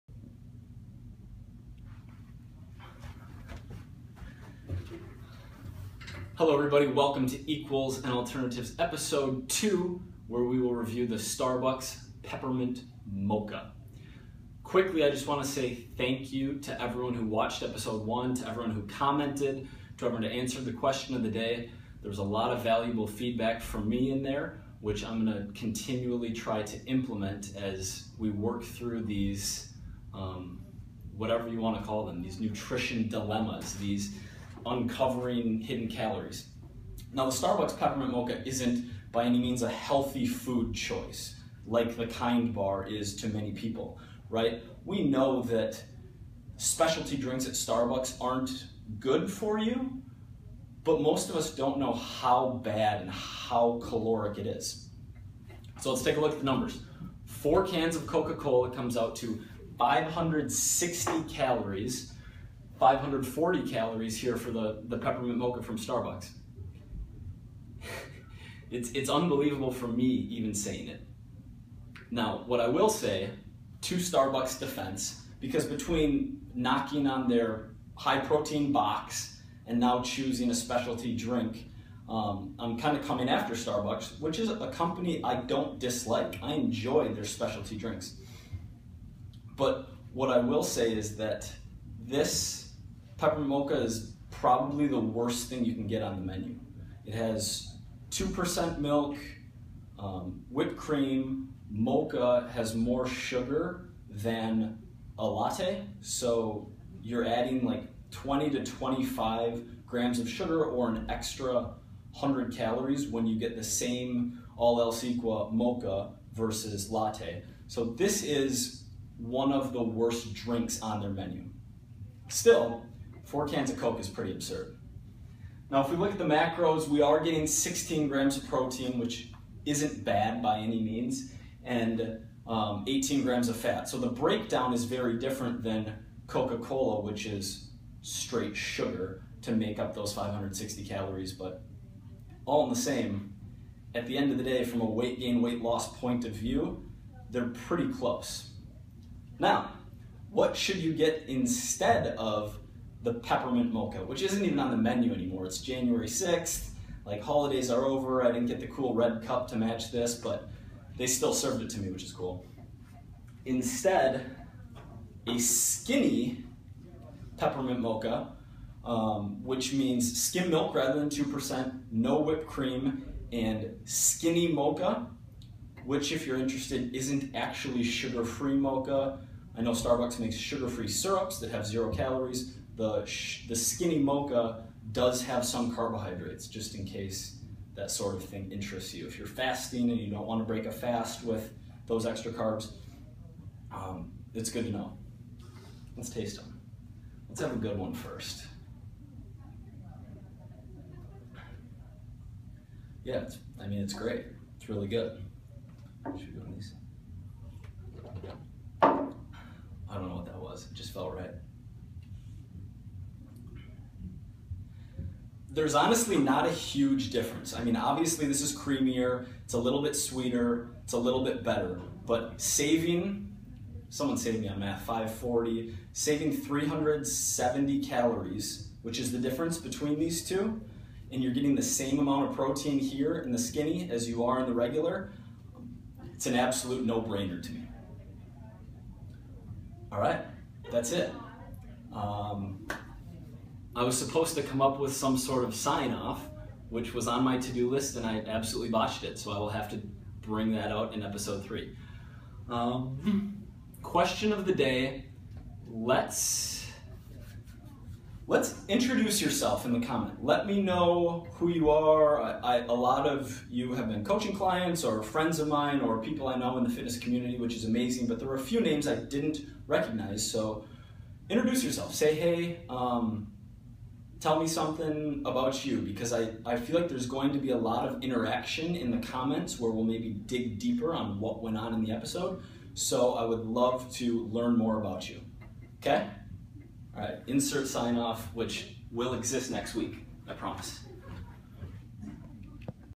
equals & alternatives Episode #2: Starbucks Peppermint Mocha
QOTD: introduce yourself! Even if we know each other, say hello so you can get to know each another too. Community is HUGE to me.

ALTERNATIVE MACROS: SKINNY PEPPERMINT MOCHA:
Protein: 16g
Carbs: 22g
Fat: 2g
Calories = 170

Mike Vacanti is a writer, entrepreneur, online fitness coach and trainer for Gary Vaynerchuk.